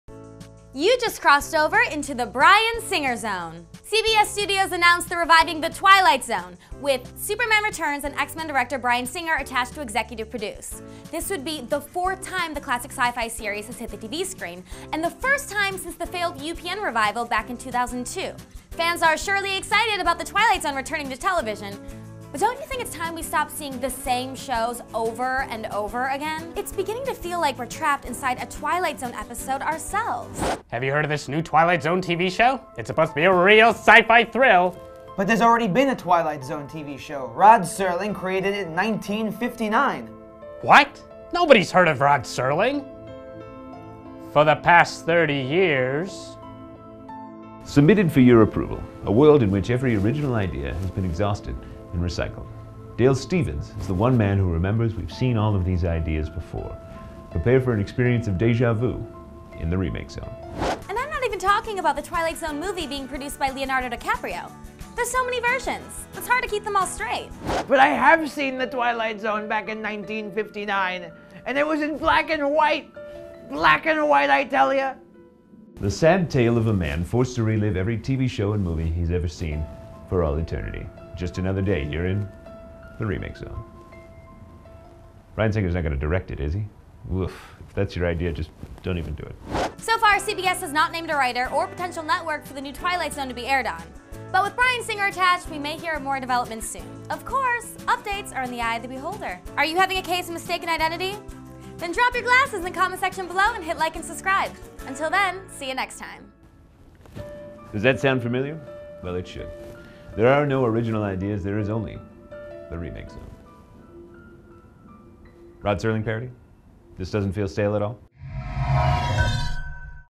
Bryan Singer To Finally Get In The Zone - The Twilight Zone
The time has come for yet another attempt at a Twilight Zone TV show revival. Let's hope it goes better than the last one. Best of luck to you, CBS.

Host: Andrea Boehlke
Cast: David Bluvband, Eugene Fomin, Jeremy Bent